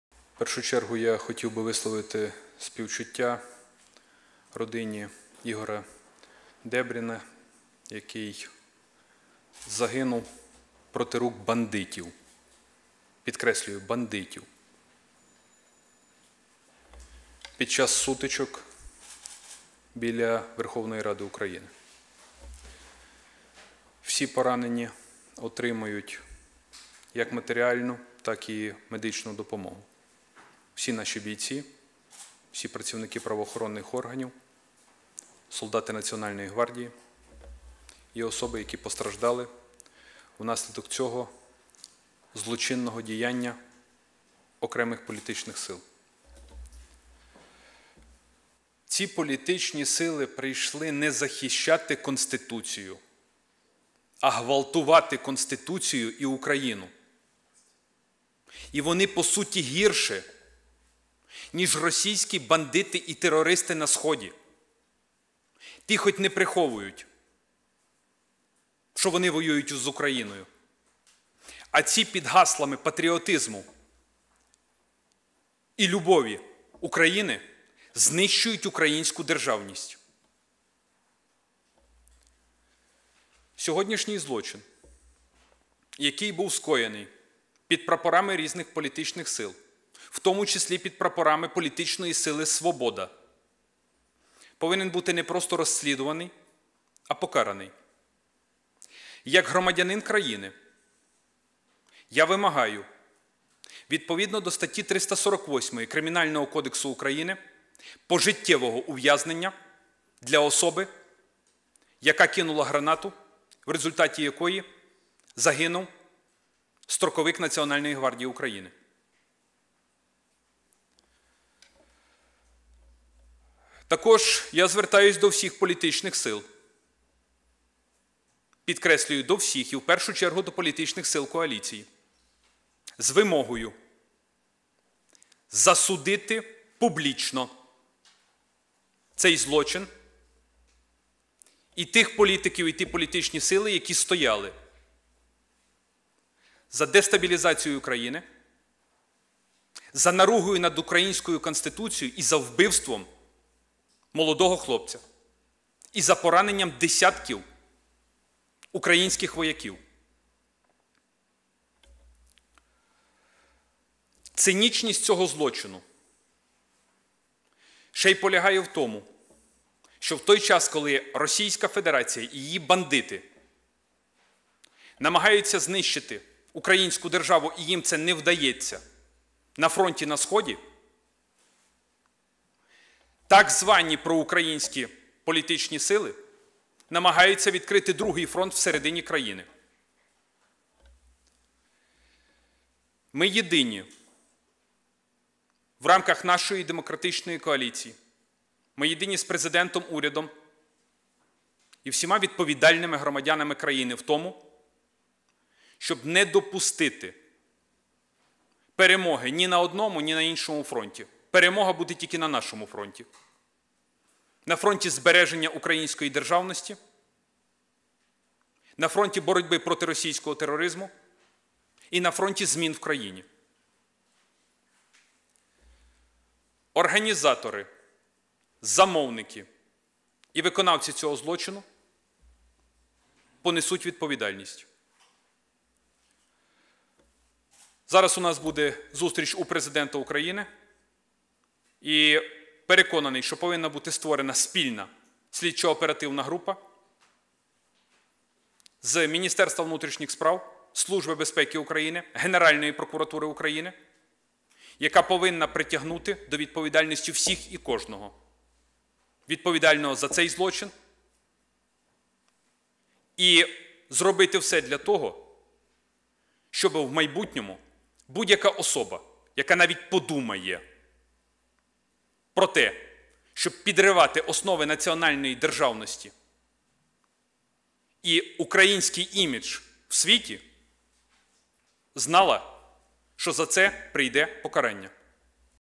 Заява Прем’єр міністра України Арсенія Яценюка
У першу чергу я хотів би висловити співчуття родині Ігоря Дебріна, який загинув від рук бандитів, підкреслюю – бандитів – під час сутичок біля Верховної Ради України.
Всі поранені отримають як матеріальну, так і медичну допомогу: всі наші бійці, всі працівники правоохоронних органів, солдати Національної гвардії і особи, які постраждали внаслідок цього злочинного діяння окремих політичних сил.
Ці політичні сили прийшли не захищати Конституцію, а ґвалтувати Конституцію і Україну. І вони, по суті, гірші, ніж російські бандити і терористи на Сході. Ті хоч не приховують, що вони воюють з Україною. А ці під гаслами патріотизму і любові до України, знищують українську державність.
Сьогоднішній злочин, який був скоєний під прапорами різних політичних сил, у тому числі під прапорами політичної сили «Свобода», повинен бути не просто розслідуваний, а покараний.
Як громадянин України я вимагаю відповідно до статті 348 Кримінального кодексу України пожиттєвого ув’язнення для особи, яка кинула гранату, в результаті чого загинув строковик Національної гвардії України.
Також я звертаюсь до всіх політичних сил, підкреслюю - до всіх і, в першу чергу політичних сил коаліції, з вимогою публічно засудити цей злочин, тих політиків і ті політичні сили, які стояли за дестабілізацією країни, за наругою над українською Конституцією, за вбивством молодого хлопця і за пораненням десятків українських вояків.
Цинічність цього злочину полягає ще й у тому, що в той час, коли Російська Федерація і її бандити намагаються знищити українську державу і їм це не вдається на фронті на Сході, так звані «проукраїнські» політичні сили намагаються відкрити другий фронт всередині країни.
Ми єдині в рамках нашої демократичної коаліції, ми єдині з Президентом, Урядом і всіма відповідальними громадянами країни в тому, щоб не допустити перемоги ні на одному, ні на іншому фронті. Перемога буде тільки на нашому фронті - на фронті збереження української державності, на фронті боротьби проти російського тероризму і на фронті змін у країні.
Організатори, замовники і виконавці цього злочину понесуть відповідальність.

Приєднуйся!